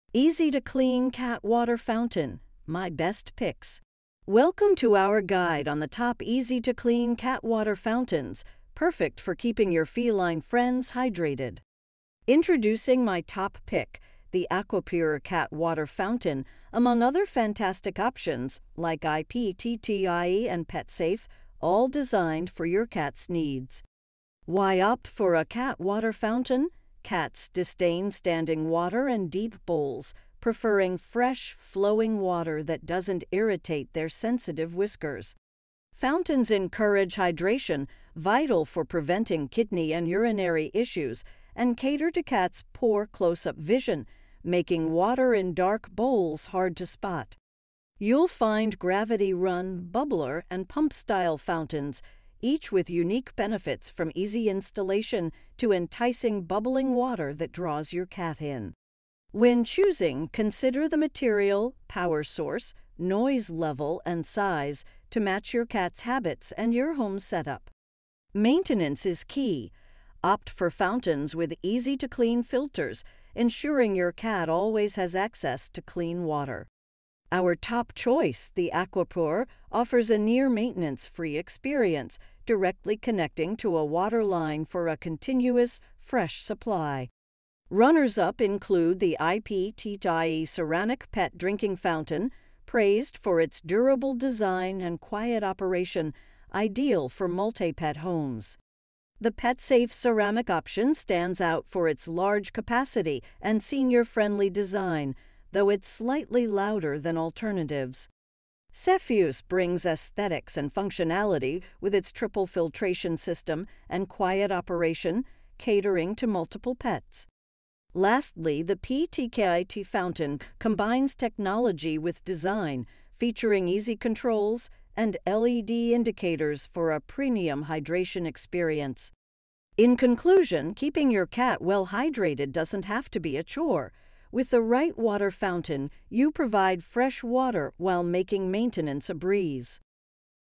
Easy-to-Clean Cat Water Fountains [Buying Guide] | Top Cat Breeds
Welcome to our guide on the best cat water fountains that make hydration a breeze for your furry friend! In this video, we'll dive into the top 5 easy-to-clean cat water fountains that not only keep your cat's water fresh but also make maintenance a snap. From sleek designs to practical features, we've curated a list that ensures both your cat's health and your convenience.